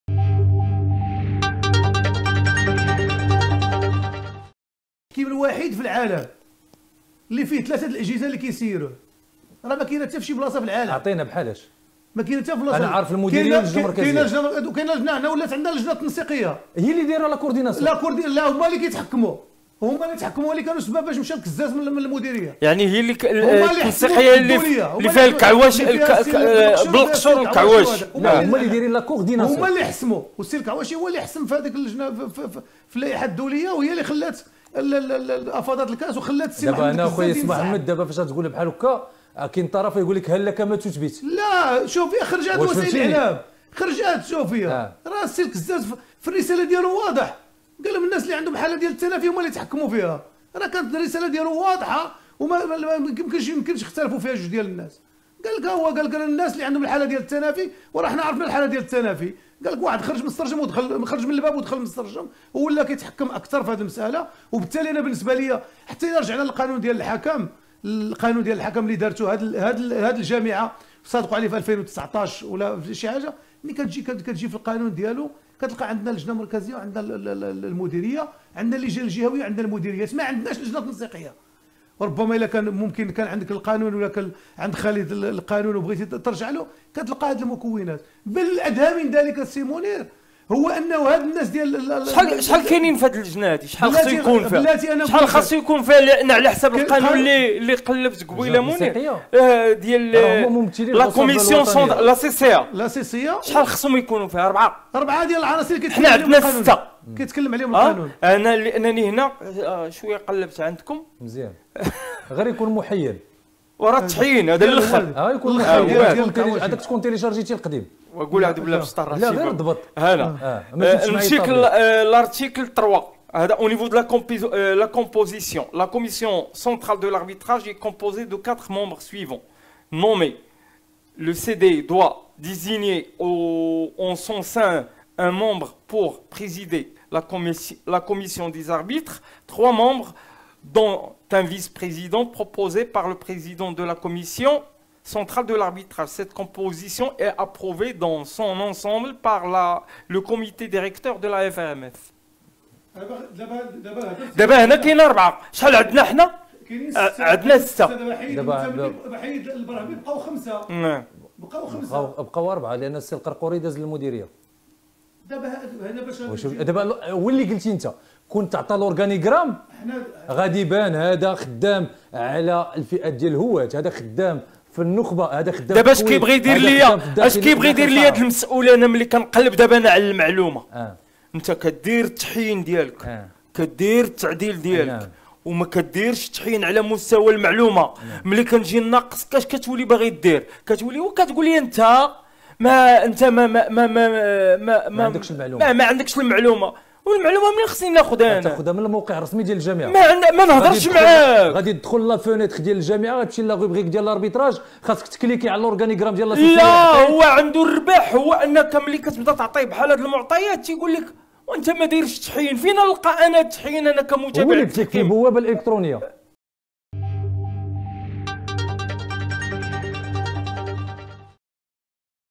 نايضة بين خبير التحكيم الموجه و بين الحكم الدولي منير مبروك بخصوص من يتحكم في التحكيم
رصدت شوف تيفي نقاش مثير خبير التحكيم الموجه و بين الحكم الدولي منير مبروك بخصوص من يتحكم في التحكيم.